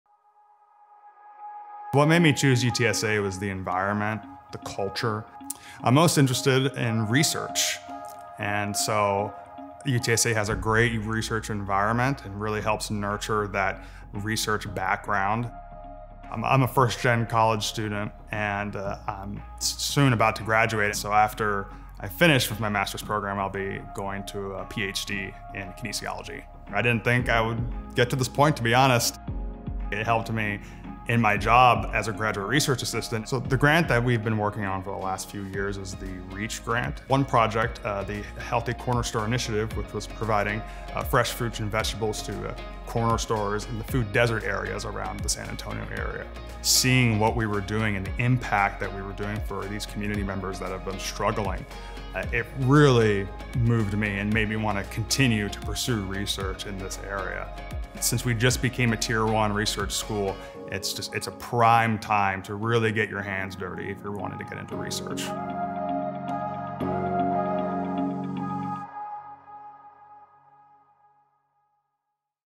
Master of Science in Kinesiology
Hear from Gabriel Impelluso as he discusses how UTSA's tier-one research and Kinesiology program enabled him to complete a master's degree and pursue his doctoral ambitions.